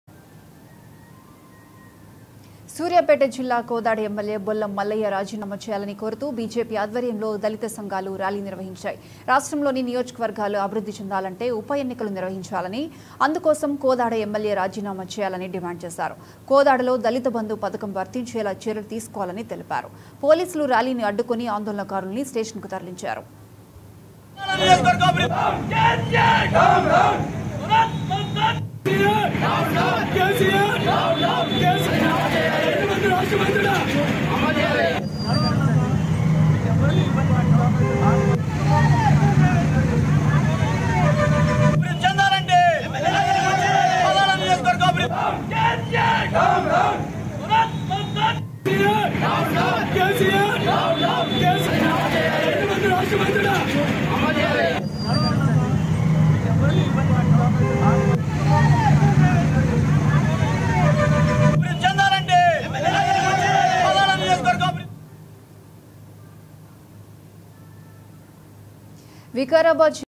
కోదాడ ఎమ్మెల్యే బొల్లం మల్లయ్య, రాజీనామా చేయాలని.. బీజేపీ ఆధ్వర్యంలో దళిత సంఘాలు ర్యాలీ| BJP | 99TV
99TV Telugu LIVE | AP News | Telangana News | 99TV Telugu Live TV Channel